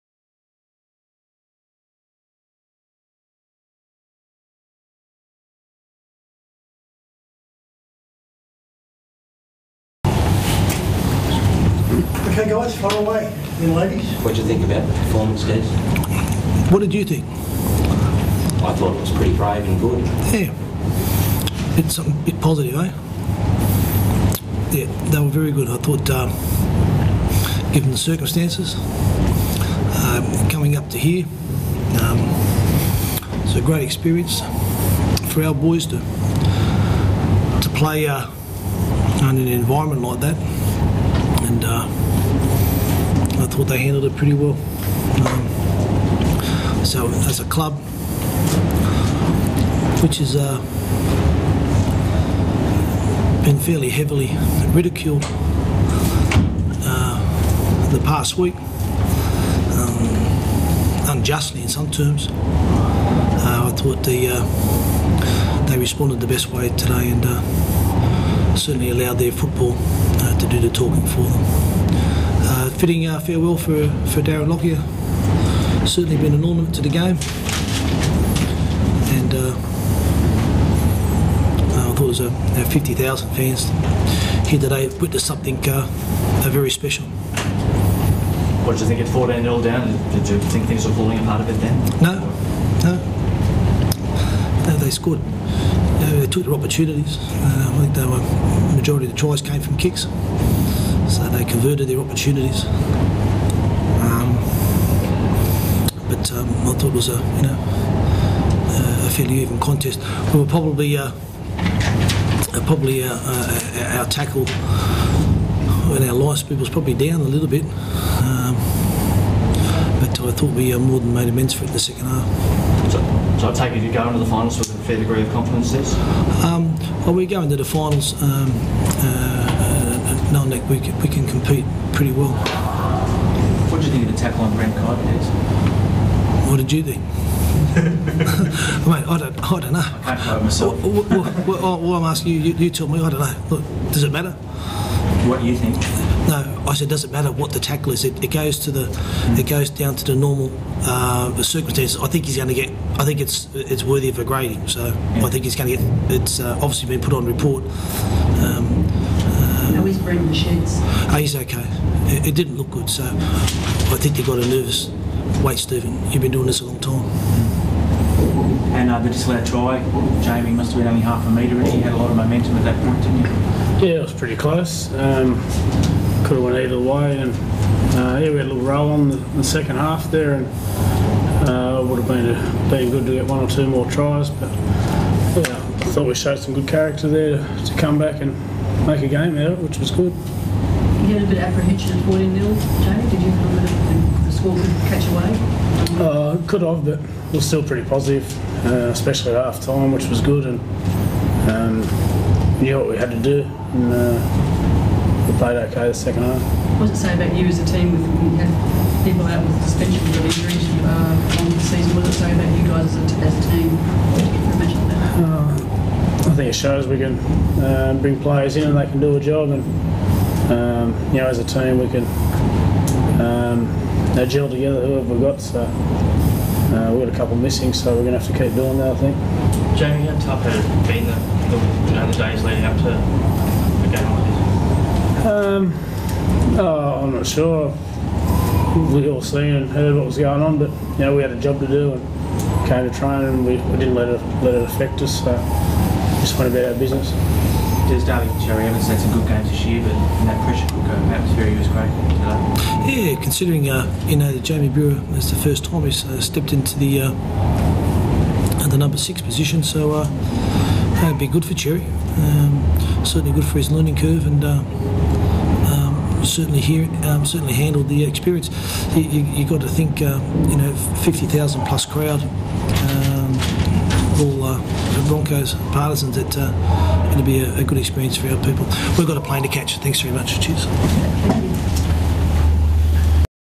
Des Hasler and Jamie Lyon
Manly Coach and Captain Broncos 18 Manly 10